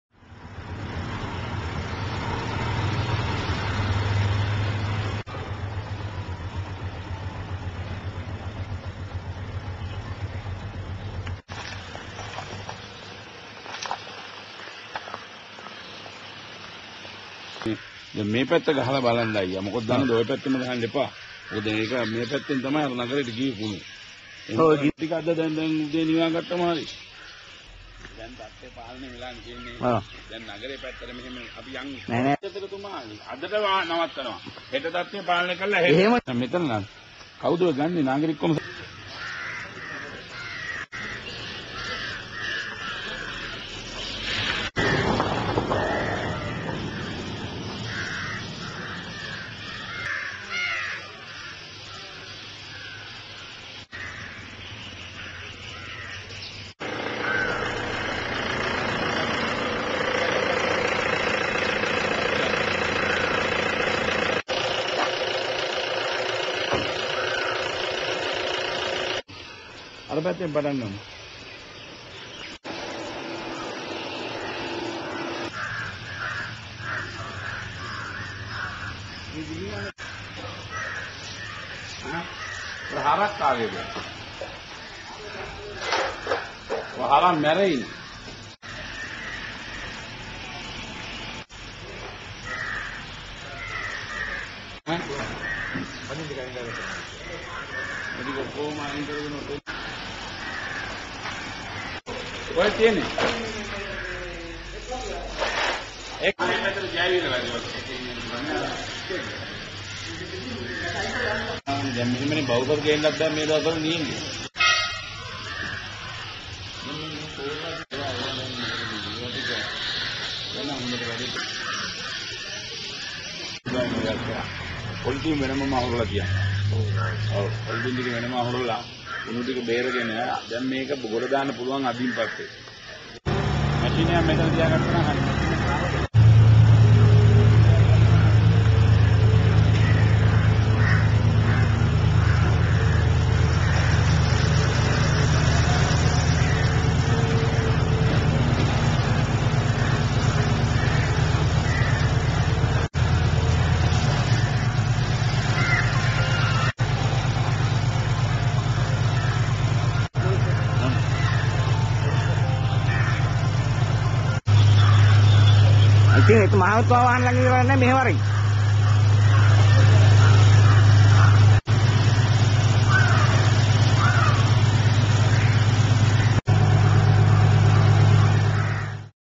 Badulla garbage dump fire continues
The  fire that erupted at a garbage dump belonging to the Badulla Municipal Council continues to rage for a third day.  Captured here is footage of the fumes emanating from the dump.